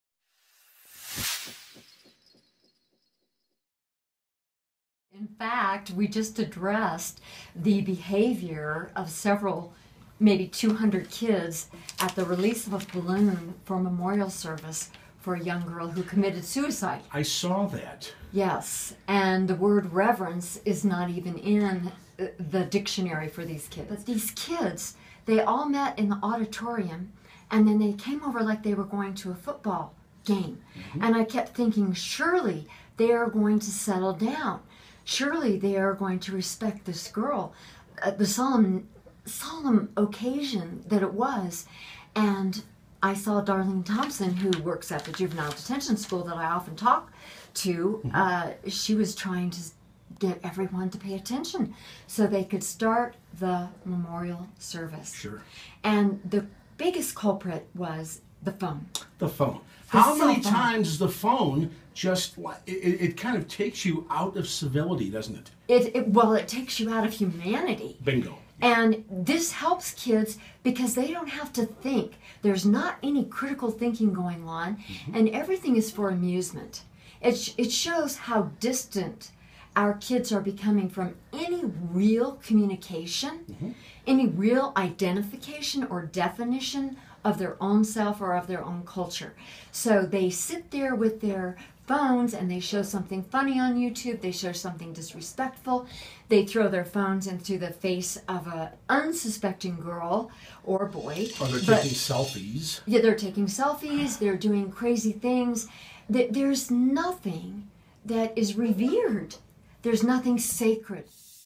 Rhonda on.... reverence and respect
Rhonda Orr describes a situation she saw in which teens at a memorial service for a girl who committed suicide acted inappropriately.